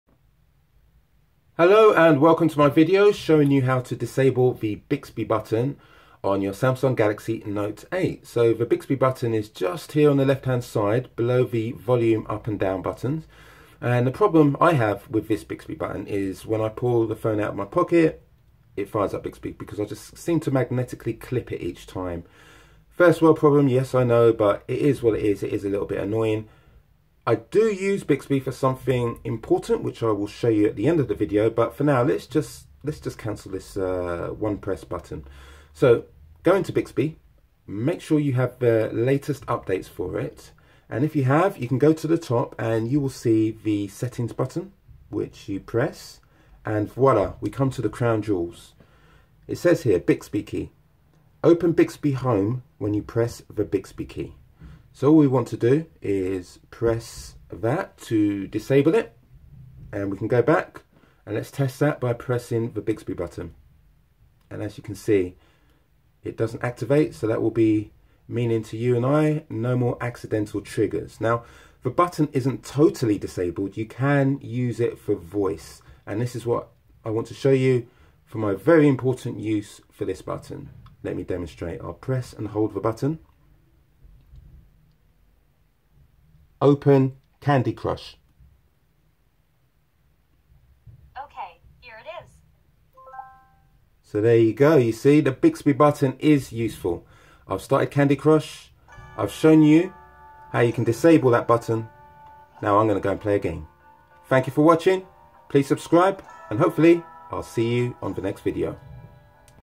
How to Disable Bixby Button - Galaxy Note 8 S8 S8+
Stop Bixby from starting everytime the button is pressed.Here's how to disable the Bixby button on your Samsung Galaxy Note 8, Samsung S8 and Samsung 8+